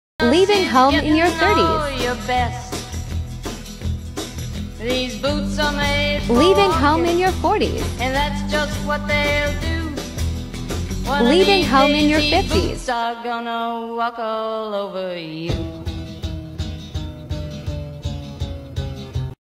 Leaving home in Your 30s - Menopause Village
Leaving home in your 30s. Shoe comfort is a pretty big deal. Yes, women in their 50ies and 60ies still wear high-heeled shoes, God Bless them! However, comfort is key and the need to not care what people think about your shoe choice is empowering.
Thank you @auntiecarols50s